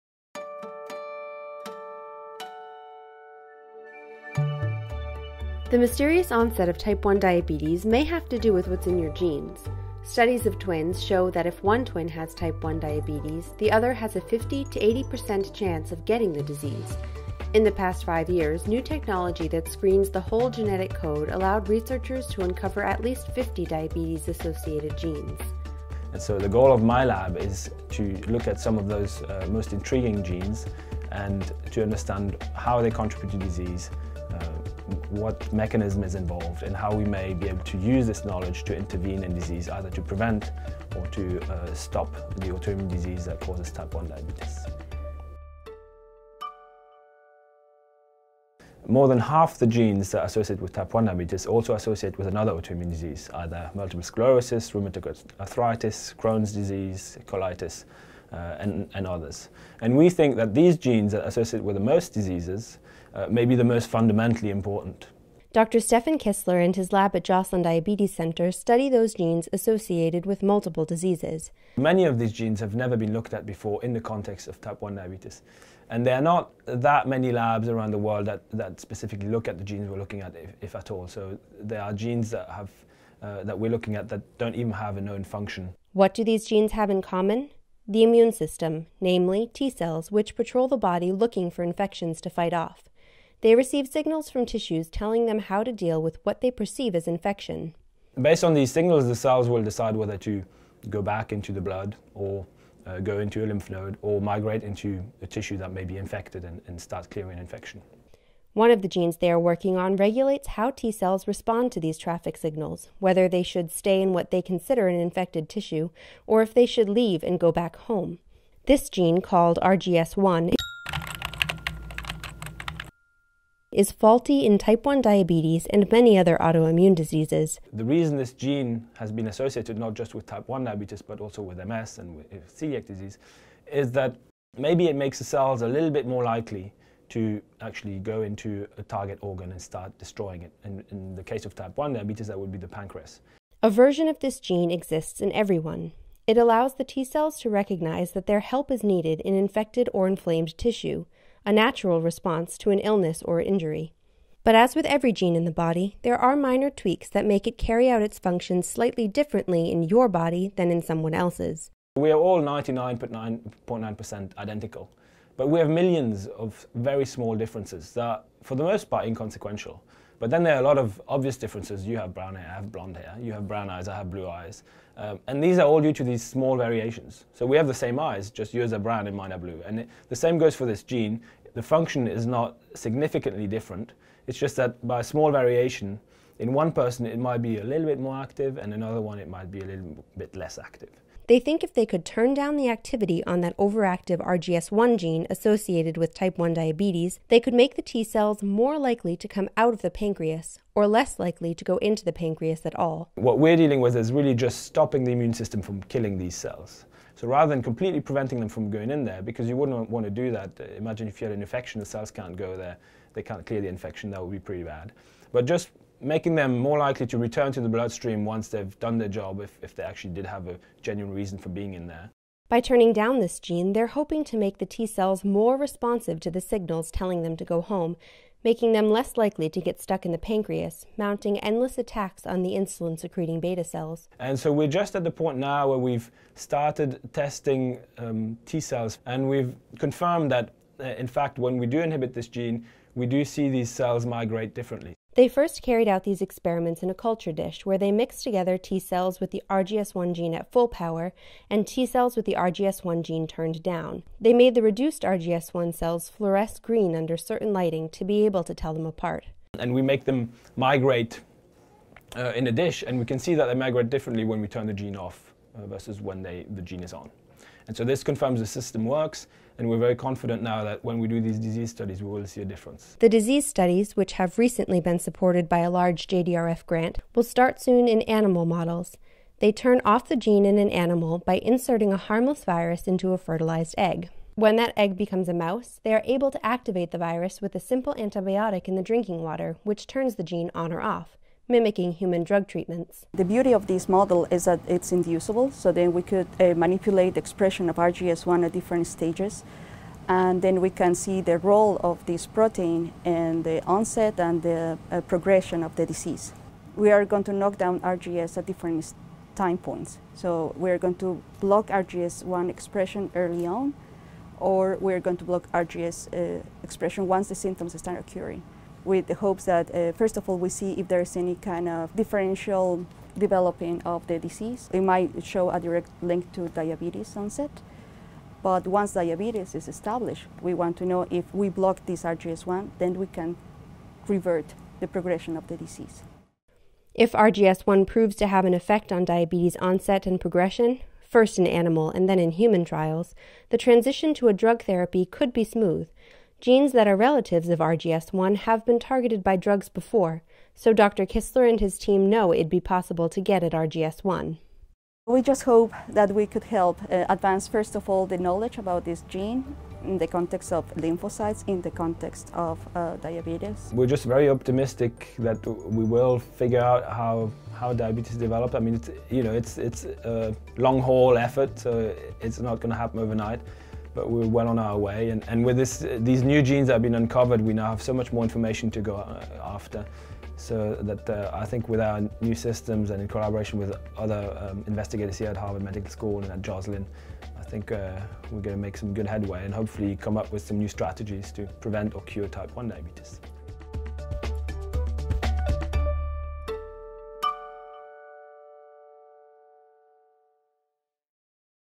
Genetics in Motion: Putting T Cells in Their Place
The mysterious onset of type 1 diabetes may have to do with what’s in your genes. Studies of twins show that if one twin has type 1 diabetes, the other has a 50 to 80 percent likelihood to also get the disease.

In the past 5 years, new technology that screens the whole genetic code allowed researchers to uncover at least 50 diabetes-associated genes. Interestingly, many of those genes also relate to other autoimmune diseases such as multiple sclerosis or celiac disease.

Stephan Kissler, Ph.D., Assistant Investigator in the Section on Immunobiology at Joslin Diabetes Center, studies those genes associated with multiple diseases using some of the newest lab techniques available.